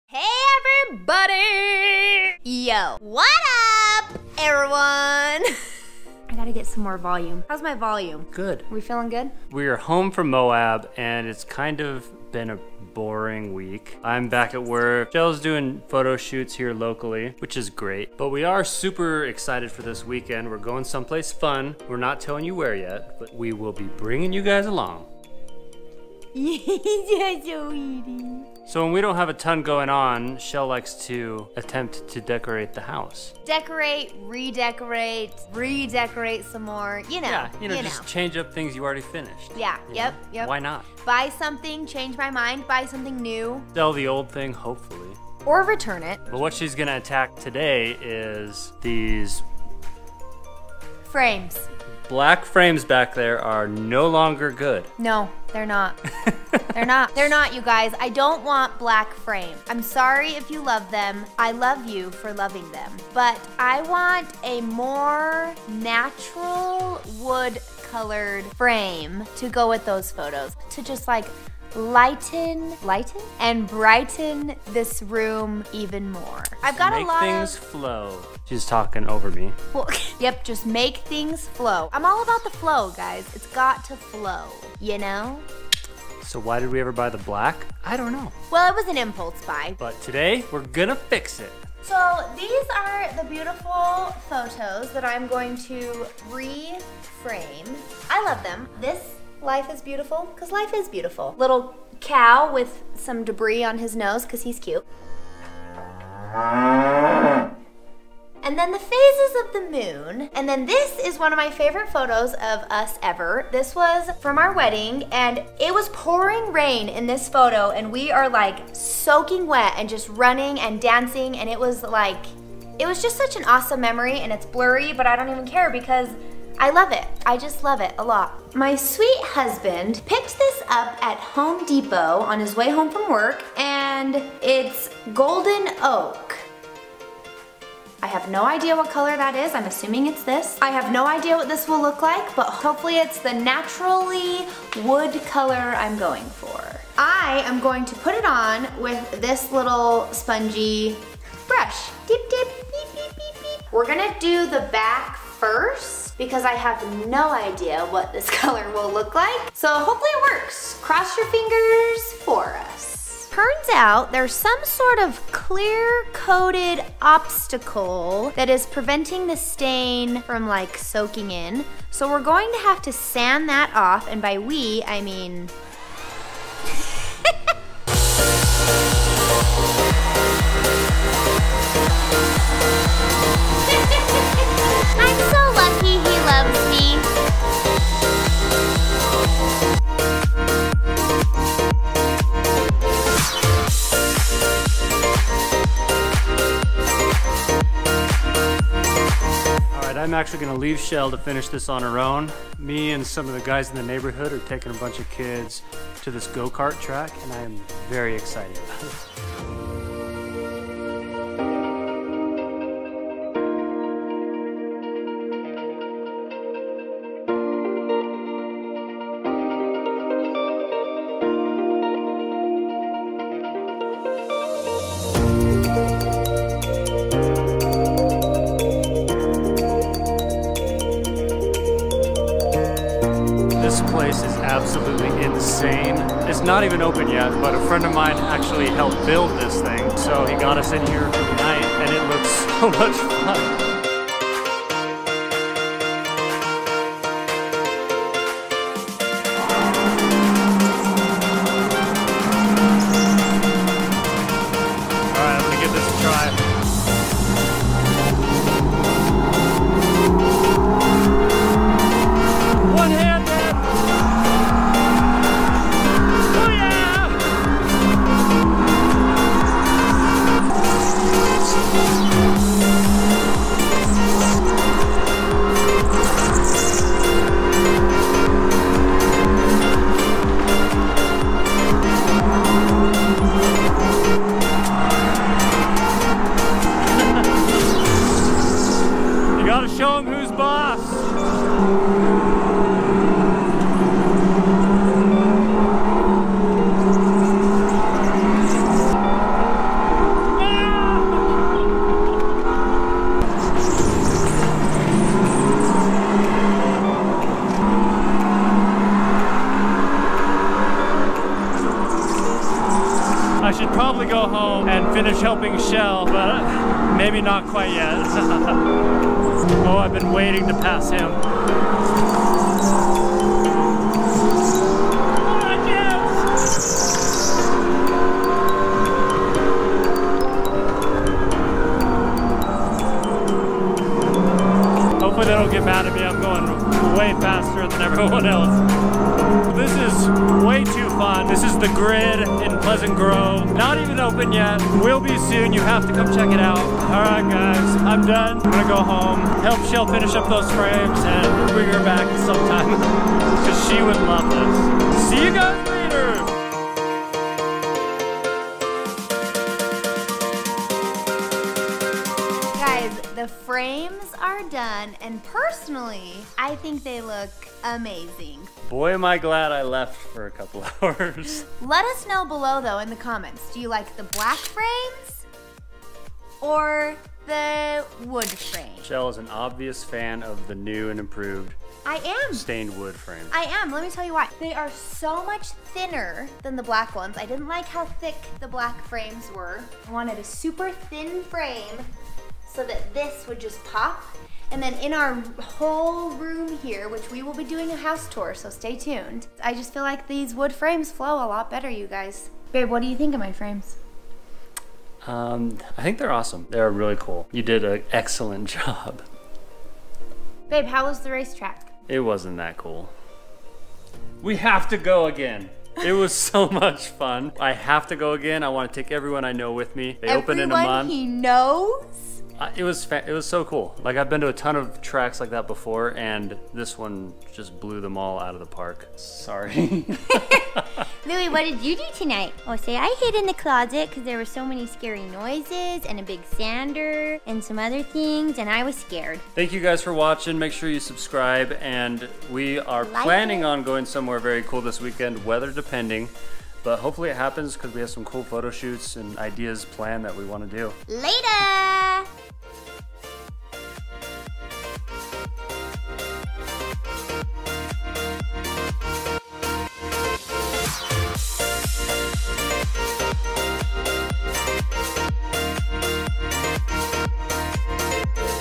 ULTIMATE RACE TRACK | INDOOR GO KARTS
Chelle and Daniel tackle some decorating around the house. Daniel also goes to race epic indoor go karts at the ultimate race track!